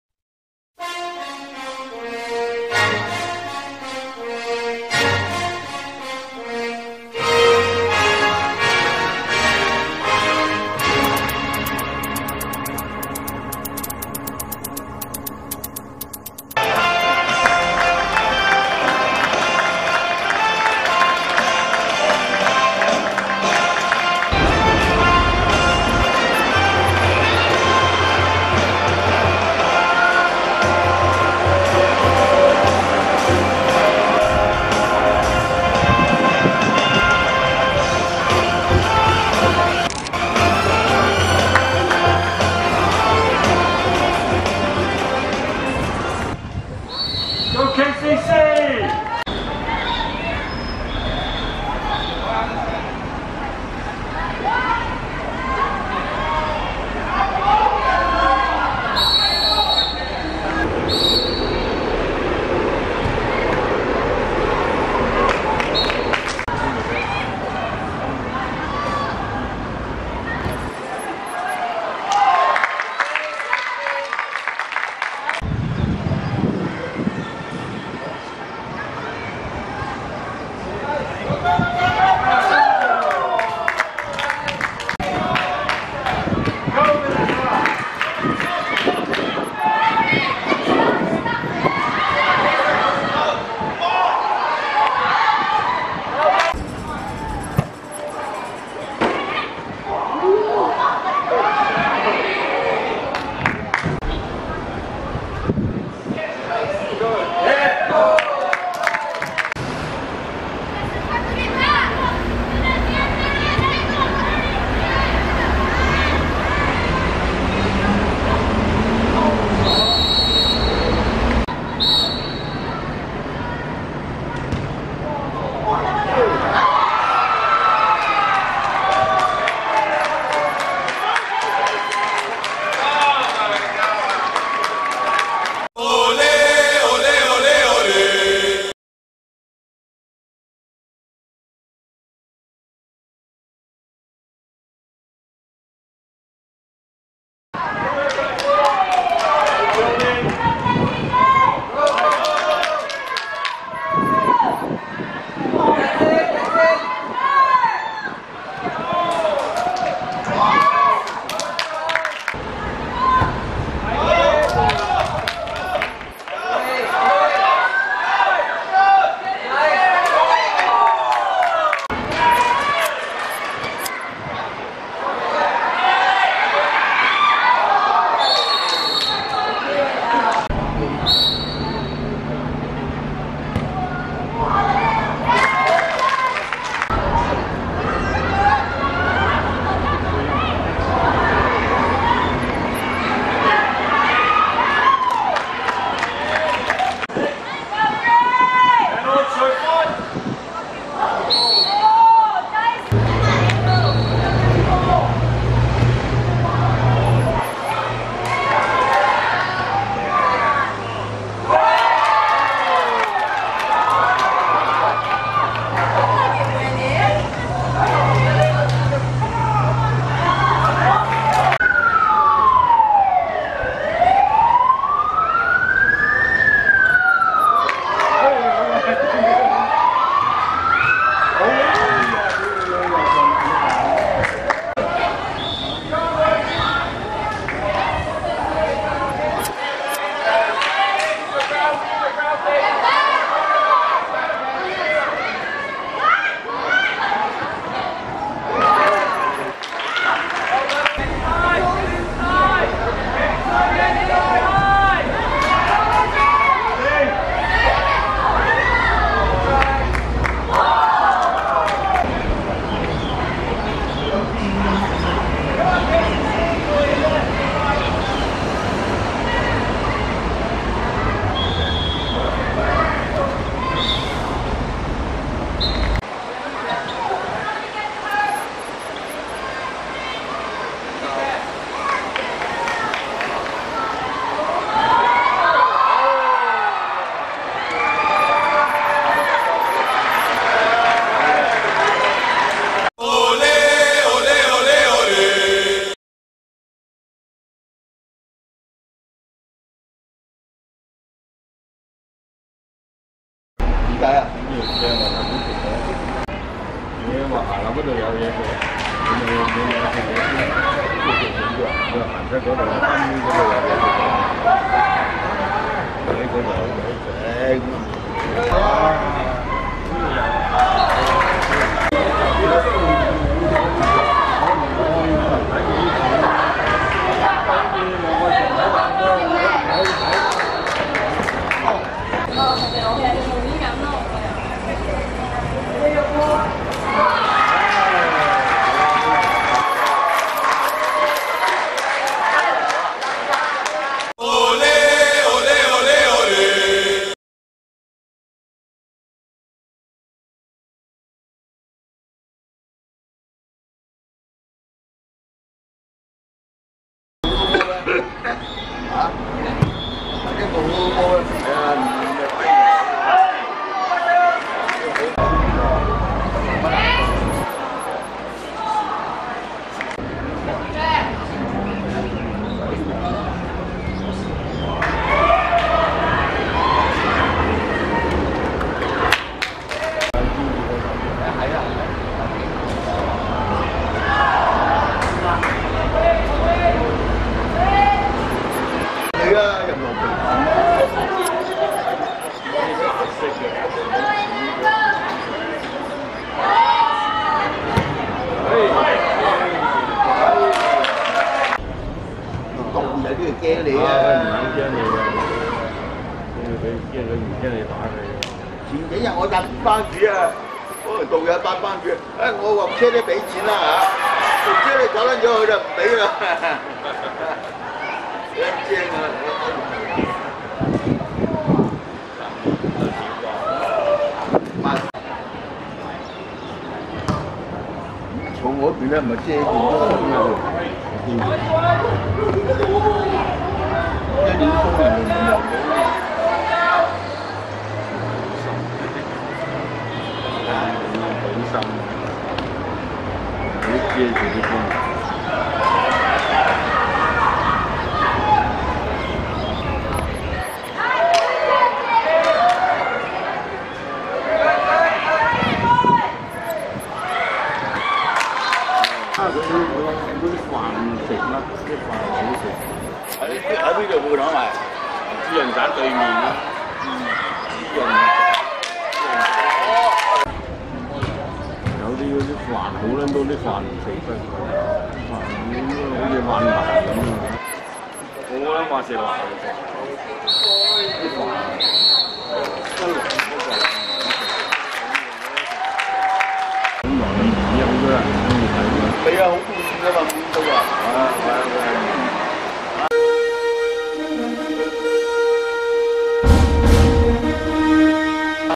大聯盟vs九龍木球會(2017.6.4.青少年女子足總盃季軍賽)精華
2017.6.4.石硤尾仿真草塲 16:00 大聯盟(紅) 3 : 0 九龍木球會(綠白間)